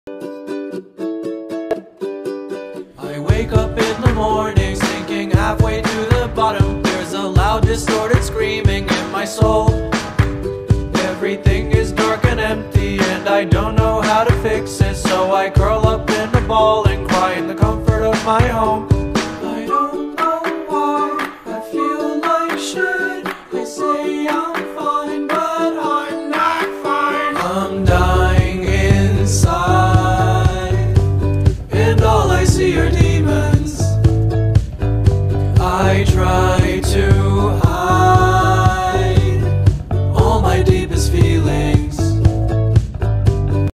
Mantelope groove.mp4
They can sing.
Not gonna lie, I liked All Tomorrows. I identify withe the mantalope a bit too much.